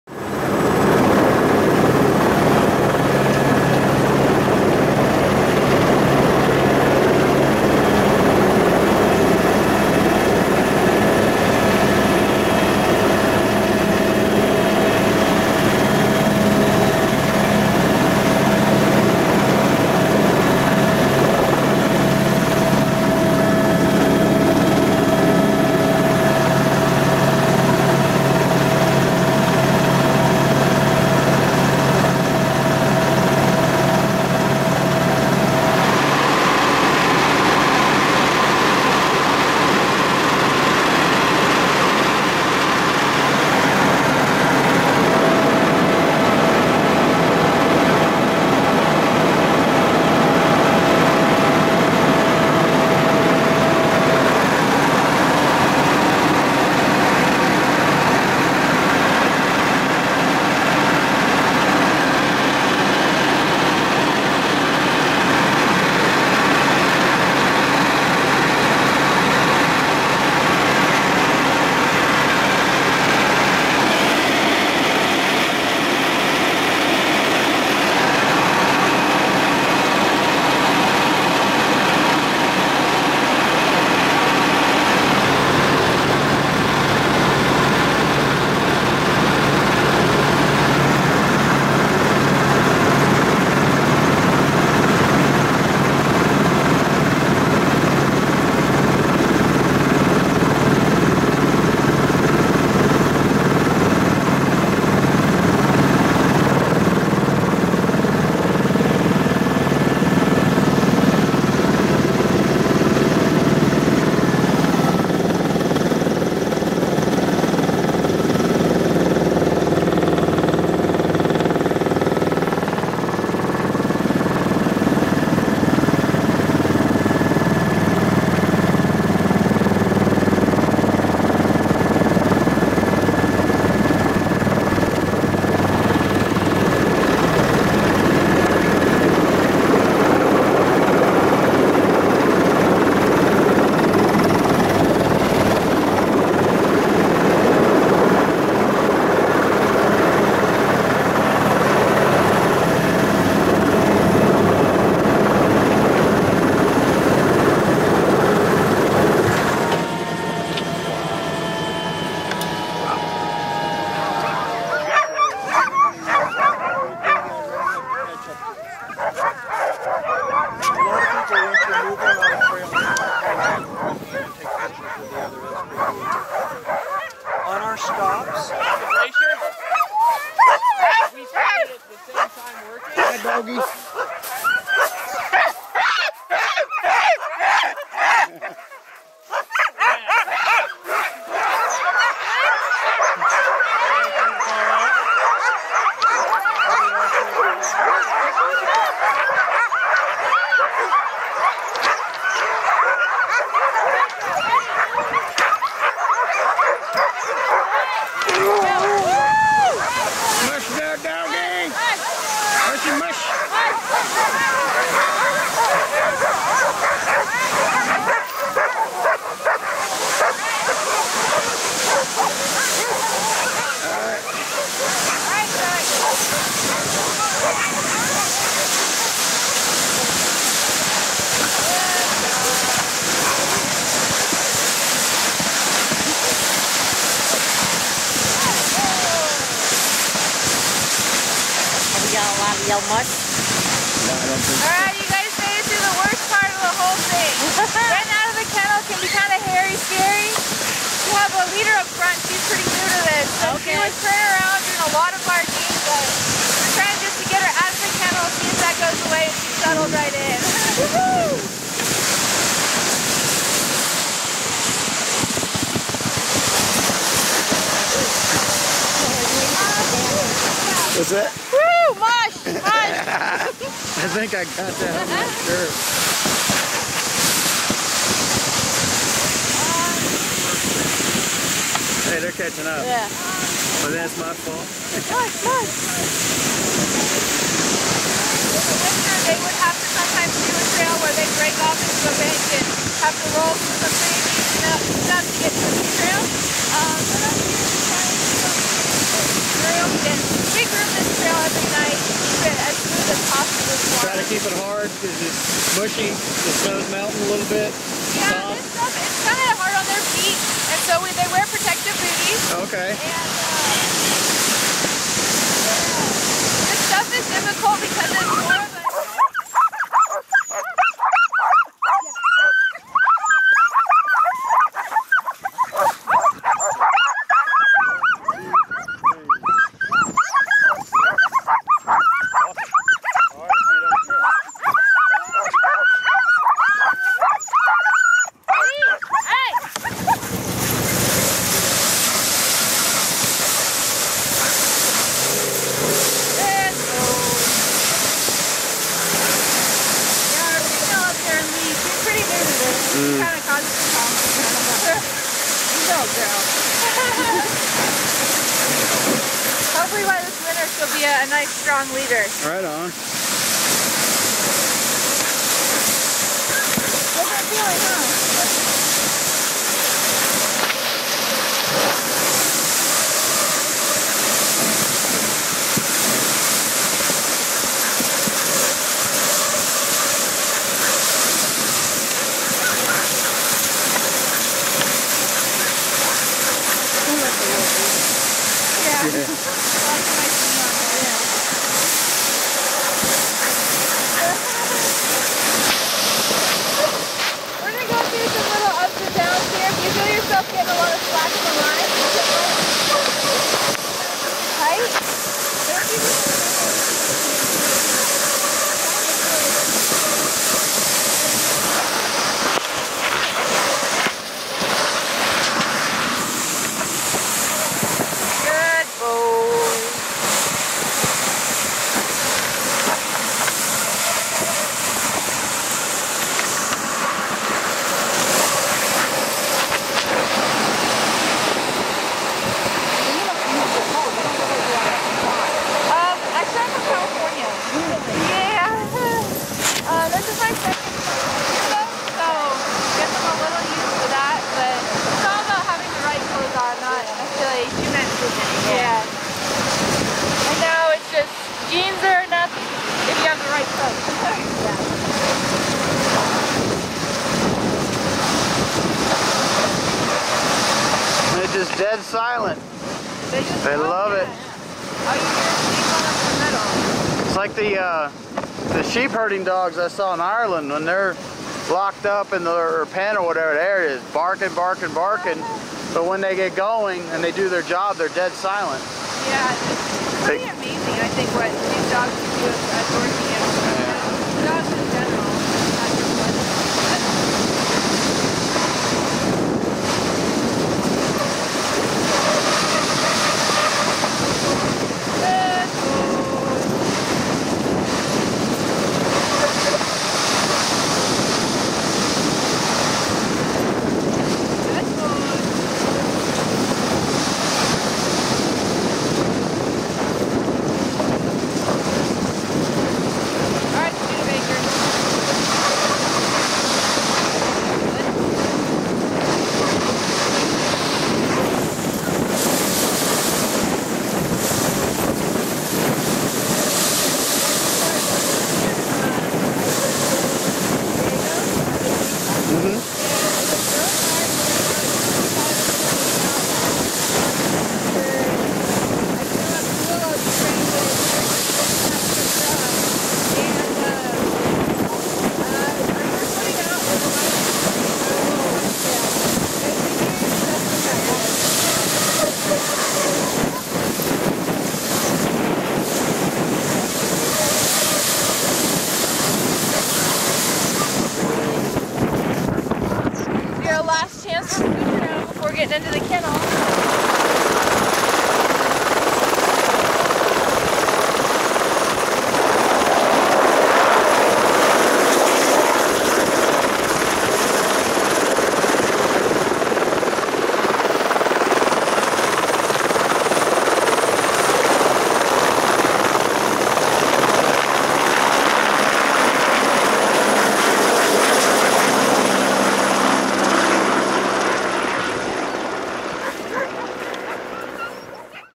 Helicopter and Dogsled ride in Alaska
This was near Juneau, Alaska. We took a chopper to the Norris Glacier and then got pulled around by the super dogs. They love to run and pull things. Note how loud they get when they have to stop and how quiet they are when they're pulling a sled. It's what they love to do. 3d version coming soon.